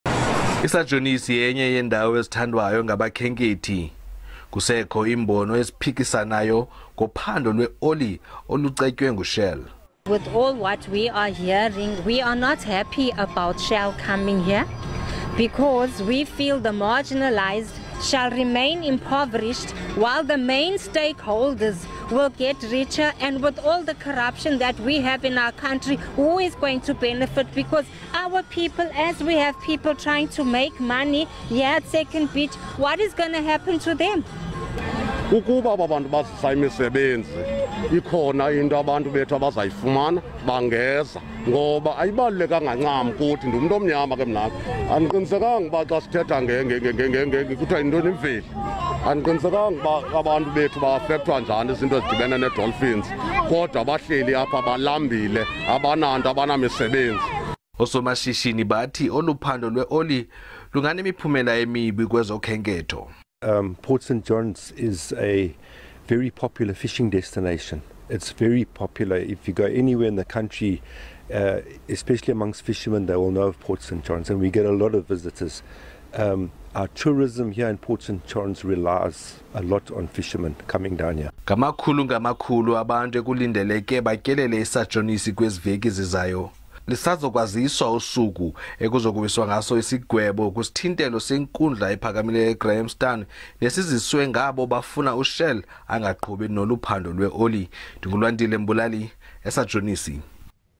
Banemibono eyehlukene ngohlelo lokuthungatha igas
Abanikazi bamabhizinisi kanye nabantu abavakashela iWild Coast ngesikhathi samaholide banemibono eyehlukene ngohlelo lokuthungatha igas lwenkampani yowoyela iShell. I-Sajonisi yaziwa njengendawo egabe ngobuhle bemvelo kanye namabhishi.  Nyakenye amabhishi alapha angamanye alawo avalwa eEstern Cape ngenkathi kuhlasele ihlandla lesithathu legciwane iCorona.